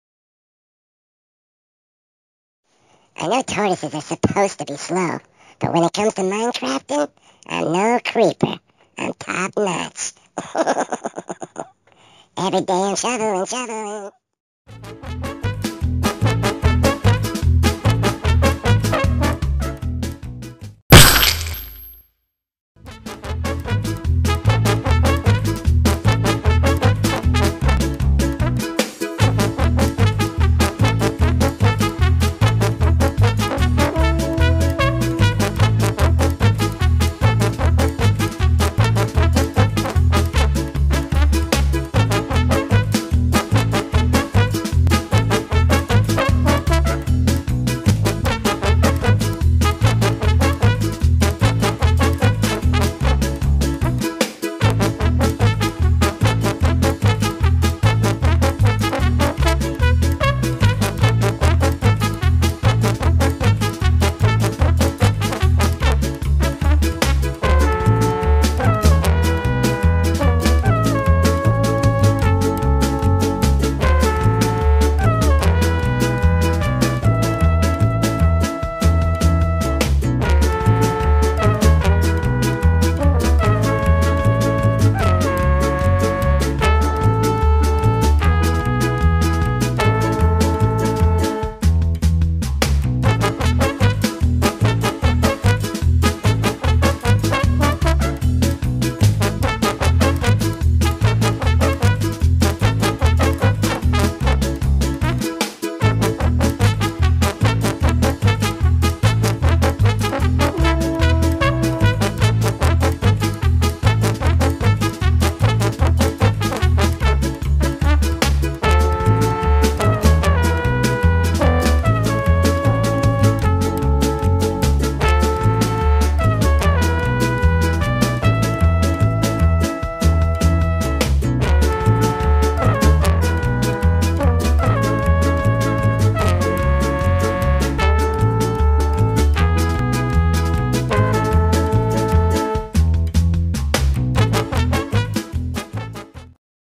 OMG!!! PET TORTOISE CRAFTS A MINECRAFT GRASS BLOCK HOUSE & ATTACKS REAL CREEPER!!! 📗🐢
Amazing pet Tortoise named Link, crafts a real life Minecraft block house for himself! But a Master builder, he is not. Seeing as how he can't even fit through the front doorway.🐢

Follow Link on Twitter @ToytubeTvT